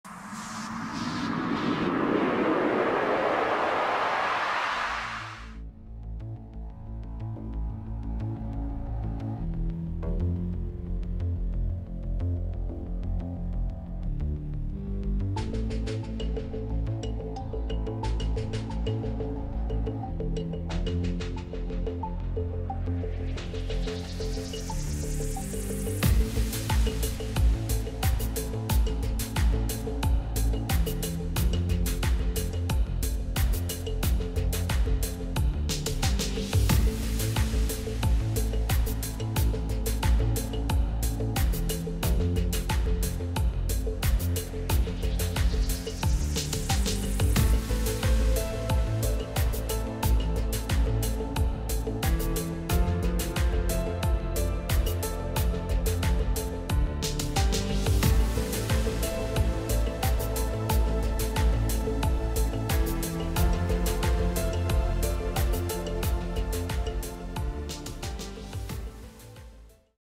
How to spray paint art : Spicy planet tutorial 5x speed
More paintings

Music
Kristian Sukari
Tuomas Pohjalainen